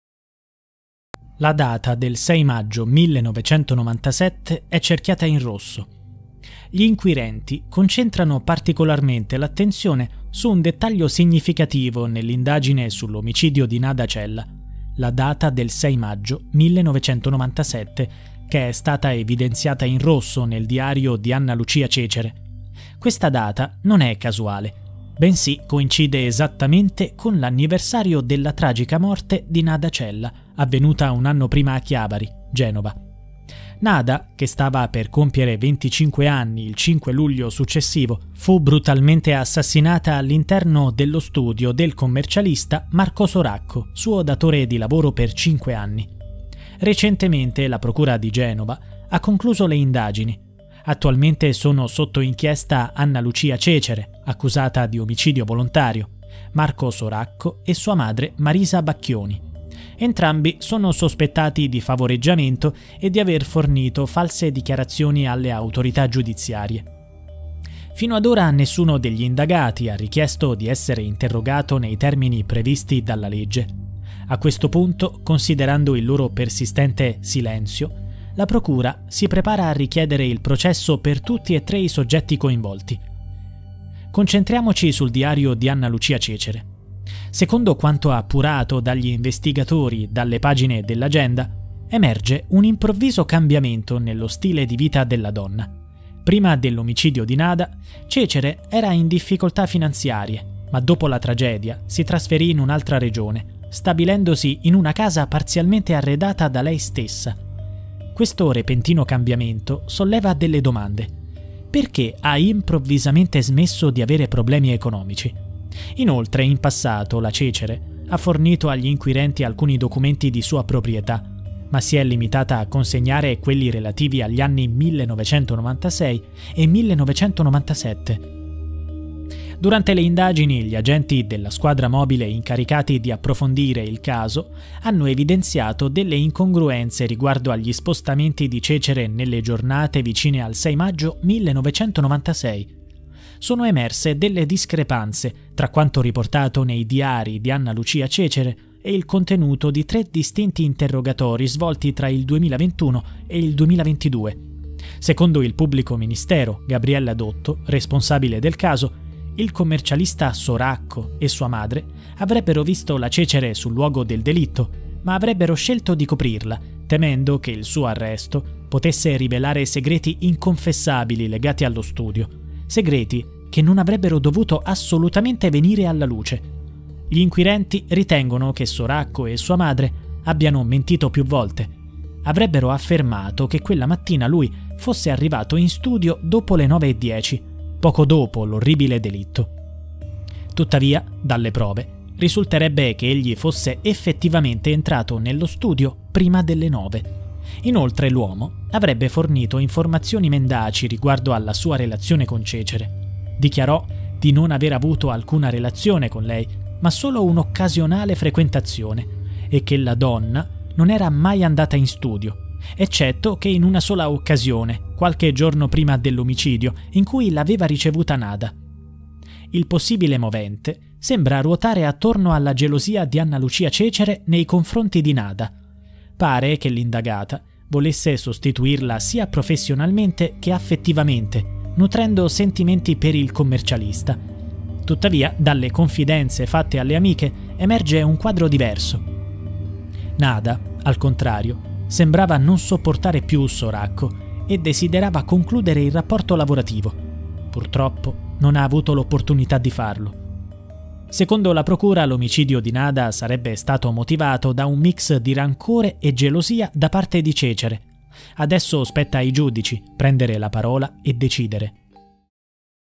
Una data sospetta - Nada Cella
Si sono chiuse con un colpo di scena le indagini sull’omicidio di Nada Cella